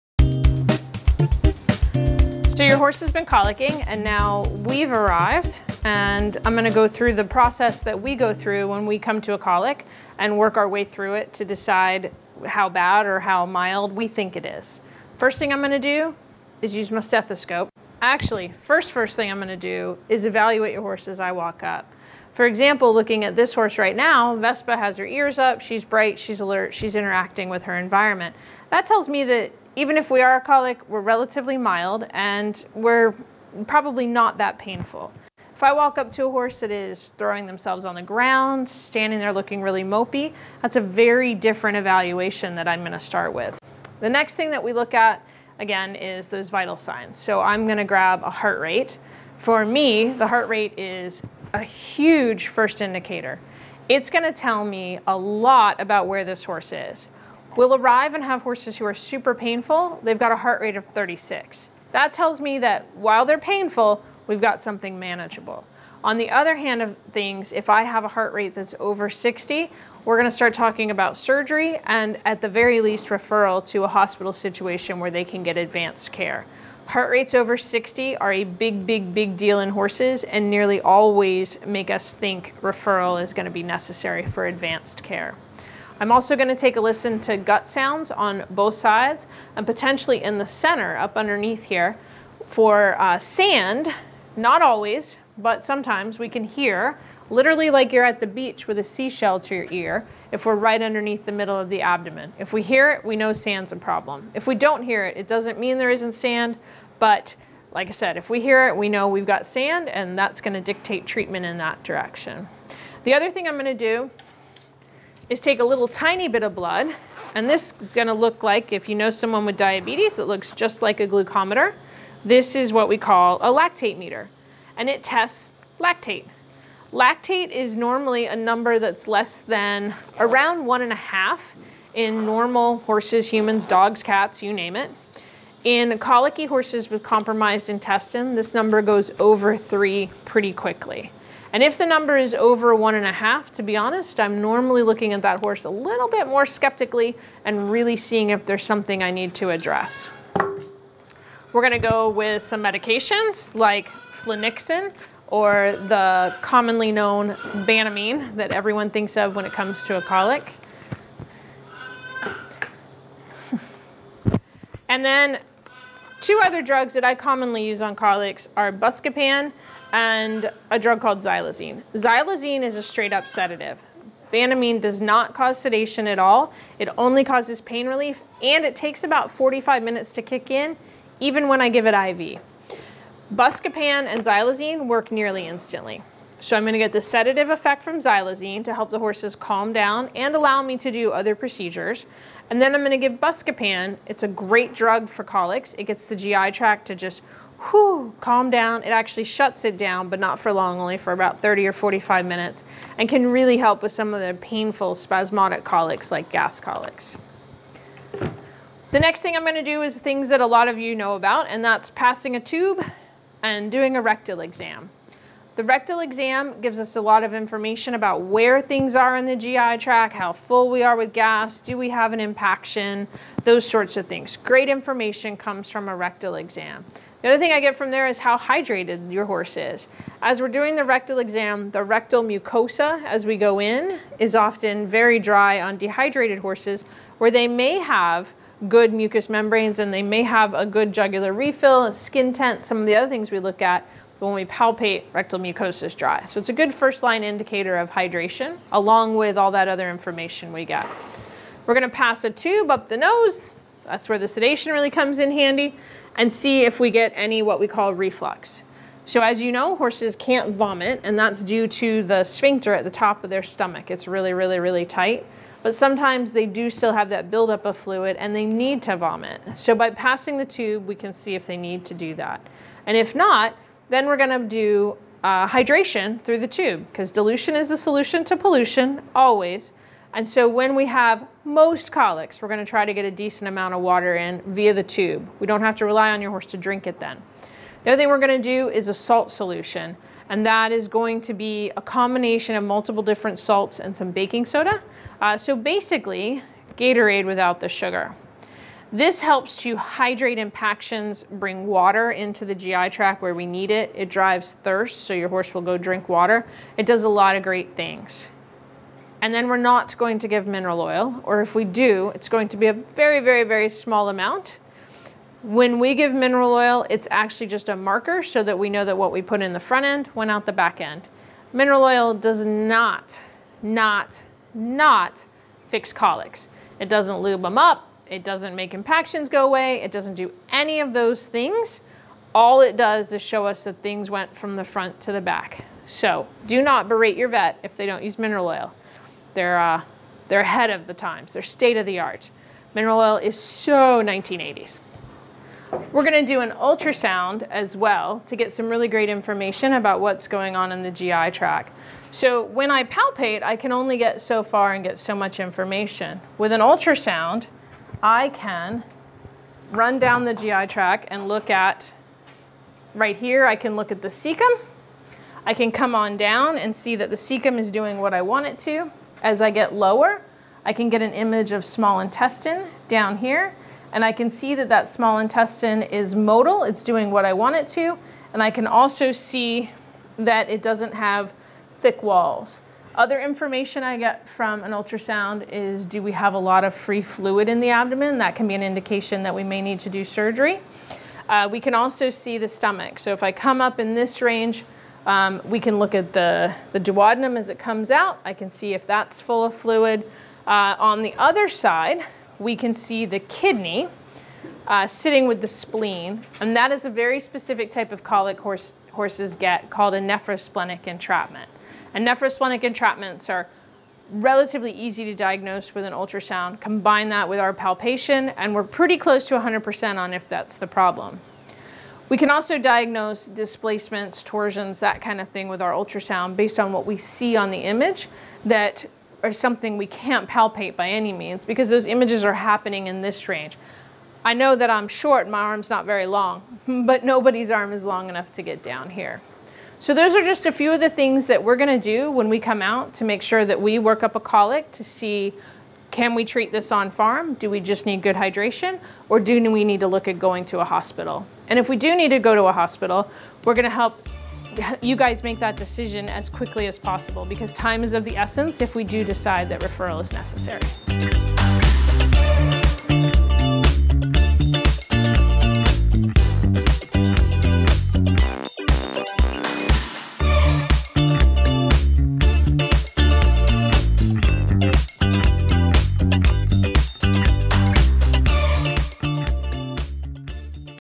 How Veterinarians Treat Colic Horses: Colic Series #3
In the 3rd video of the four-part Colic Series, Dr. Erica Lacher of Springhill Equine Veterinary Clinic explains the processes and tools that veterinarians can use to diagnose and treat colic horses.